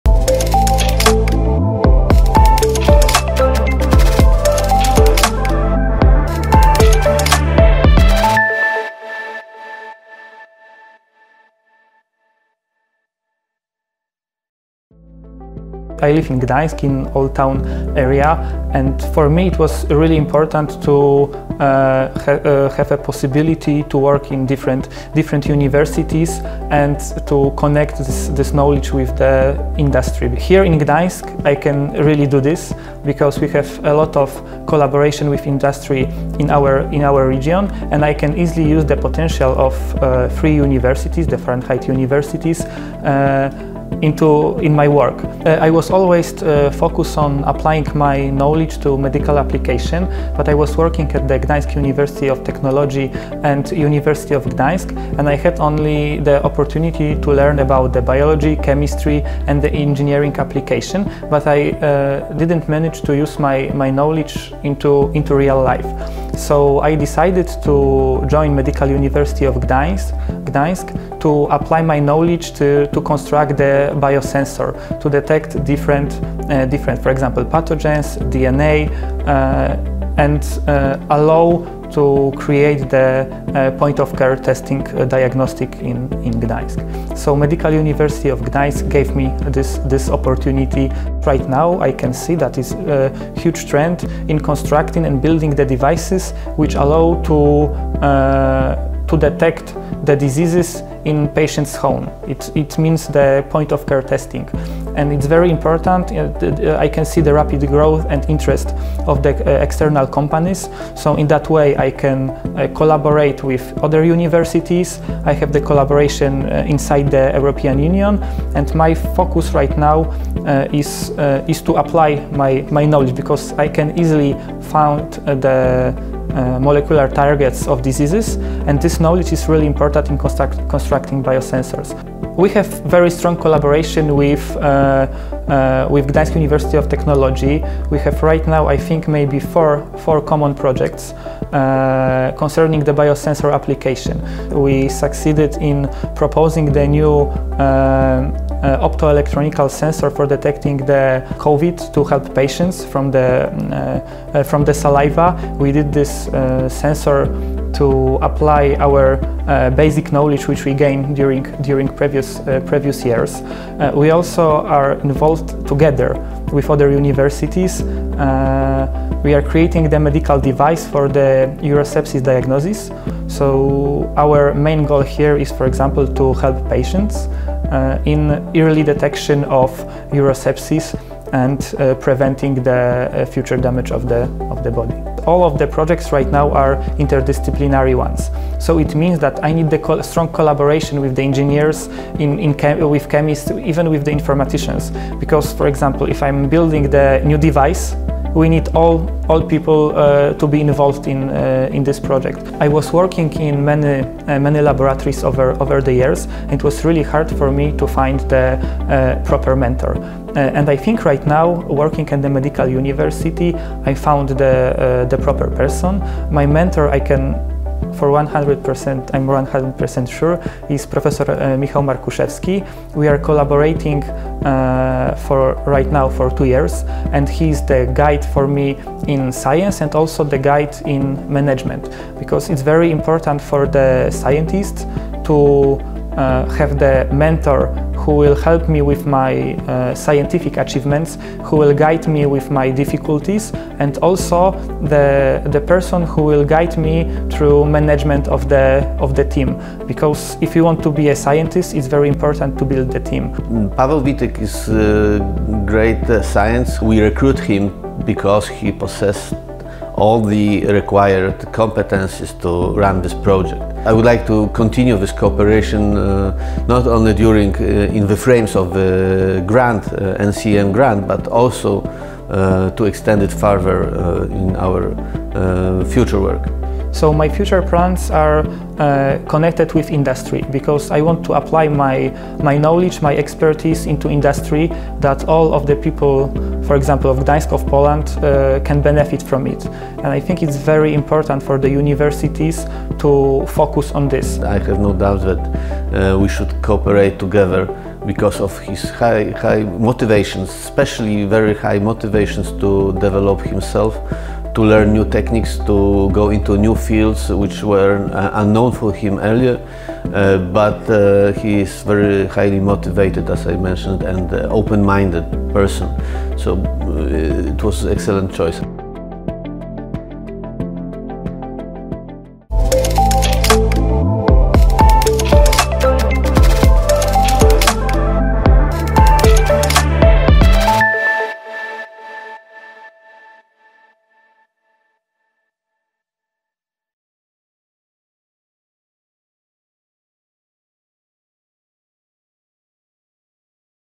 Developing advanced biosensors and modern point-of-care testing methods
Applying engineering principles within medicine and healthcare is no easy task. Yet, one of major importance. It  facilitates the development of new innovations and thus enhances and saves the patients’ lives more efficiently.

Follow Paweł Wityk, Ph.D. Eng., whose major aim is to construct biosensors to detect various pathogens – such as, e.g. an optoelectronic sensor for detecting SARS CoV-2 infection from saliva. He hopes that it will help in the development of modern point-of-care testing.

Dr. Wityk’s second major project involves the work on constructing a new device for urosepsis diagnosis.

Learn how the Fahrenheit Union of Universities in Gdańsk aids him in his scientific endeavours and experience the value of true mentorship.